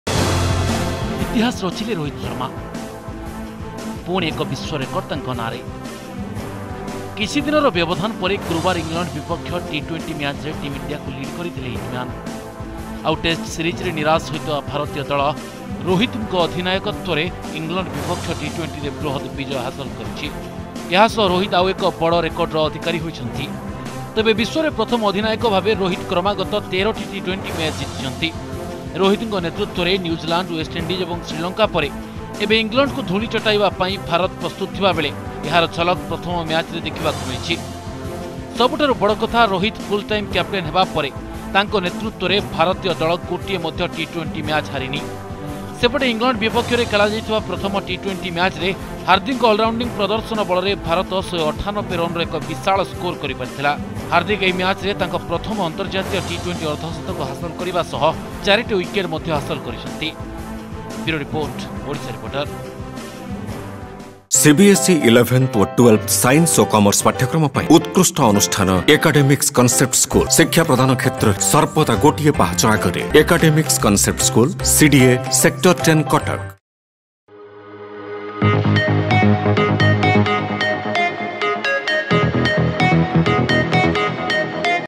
ଯାହା ବିରାଟ୍ କରିନଥିଲେ, ରୋହିତ କରି ଦେଖାଇଲେ...| Odisha Reporter
ଇତିହାସ ଚରିଲେ ରୋହିତ ଶର୍ମା। ପୁଣି ଏକ ବିଶ୍ୱ ରେକର୍ଡ ତାଙ୍କ ନାଁରେ। କିଛି ଦିନର ବ୍ୟବଧାନ ପରେ ଗୁରୁବାର ଇଂଲଣ୍ଡ ବିପକ୍ଷ ଟି-20 ମ୍ୟାଚରେ ଟିମ୍ ଇଣ୍ଡିଆକୁ ଲିଡ୍ କରିଥିଲେ ହିଟ-ମ୍ୟାନ। ଆଉ ଏହି ମ୍ୟାଚ ଭାରତ ଜିତିବା ସହ ରୋହିତଙ୍କ ନାଁରେ ଆଉ ଏକ ବିଶ୍ୱ ରେକର୍ଡ ସାମିଲ ହୋଇଛି.